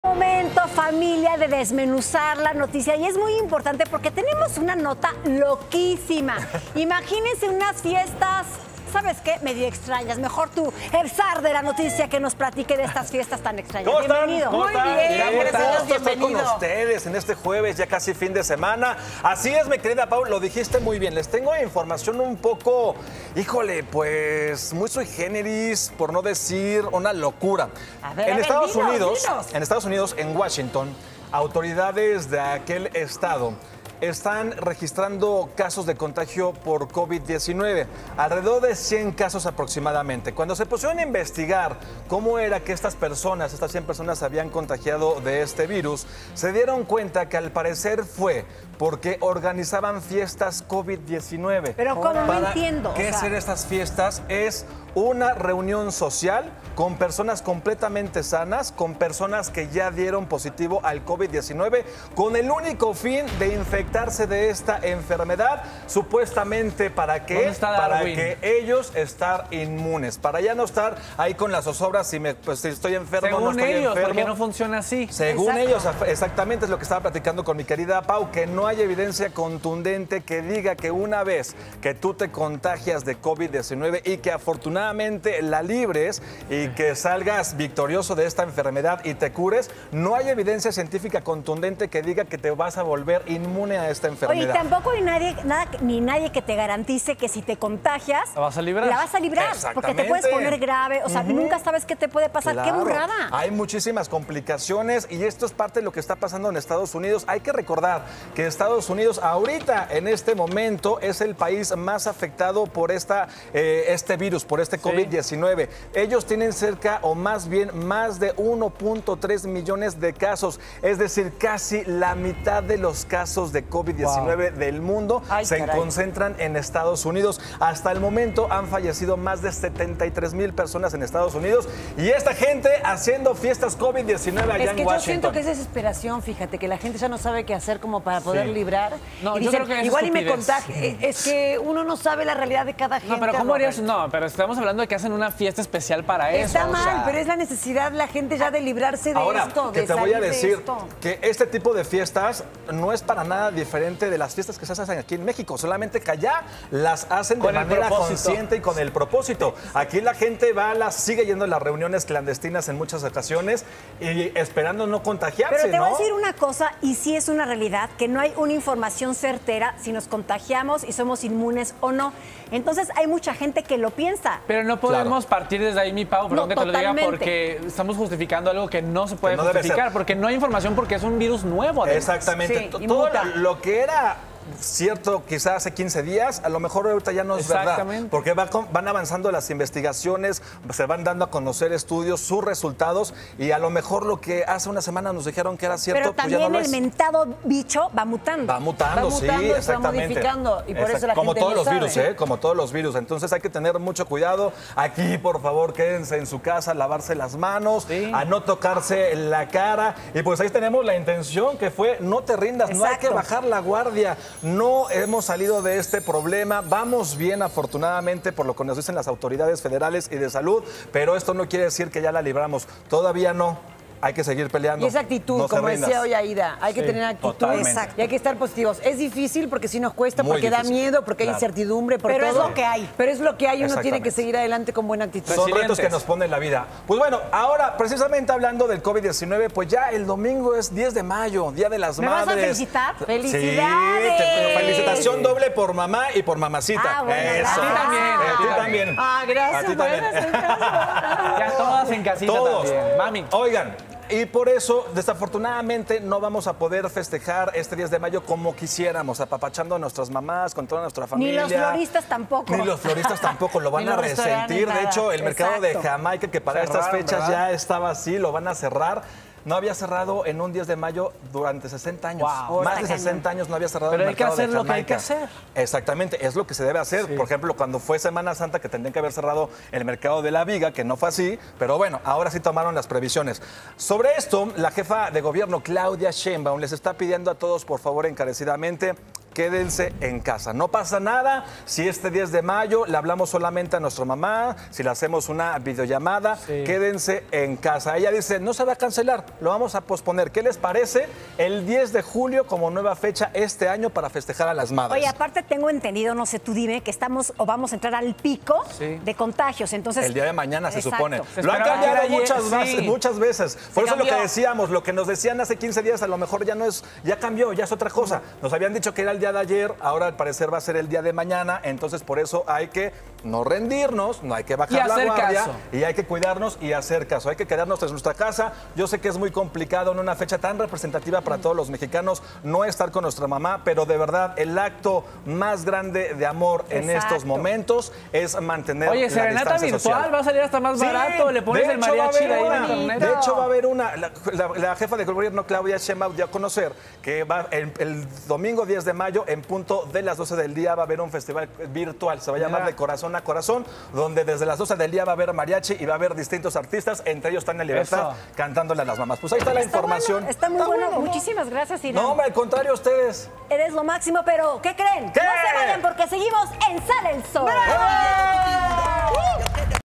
¡Jóvenes organizan 'Fiestas COVID-19' para contagiarse!
HiramHurtado nos trae más noticias. Varios jóvenes se han dedicado a hacer llamadas 'Fiestas COVID-19' para contagiarse o volverse inmunes.